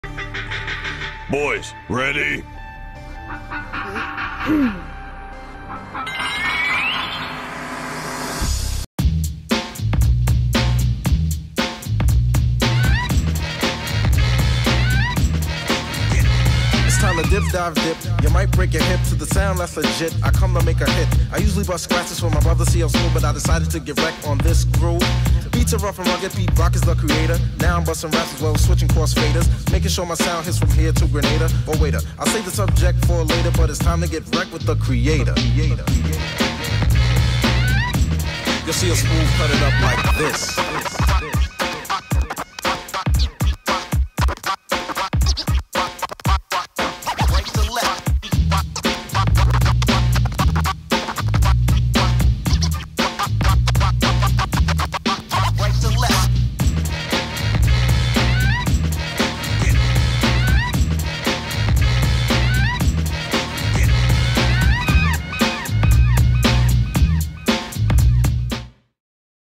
Android: Handshake Parody
Android Commercial Parody saw the commercial yesterday thought it would be cool to remix it